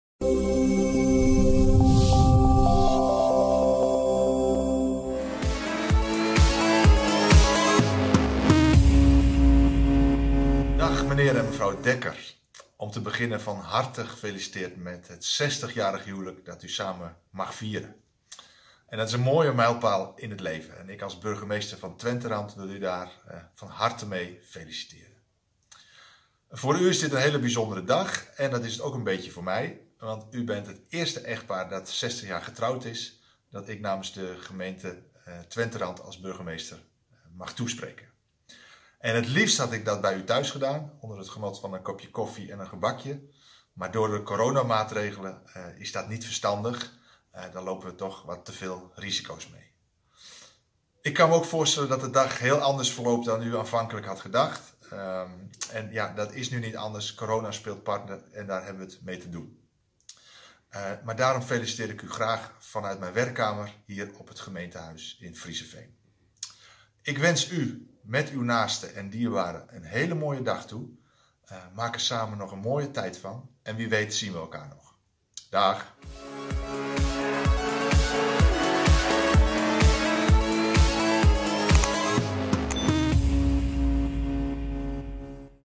VRIEZENVEEN: 60-jarig huwelijksjubileum Mw & Dhr. Dekker-Fokke 🦠
Persoonlijke felicitatie van burgemeester Broekhuizen voor het diamanten echtpaar Dekker-Fokke.

en via Internet op alle apparaten op of via Instagram.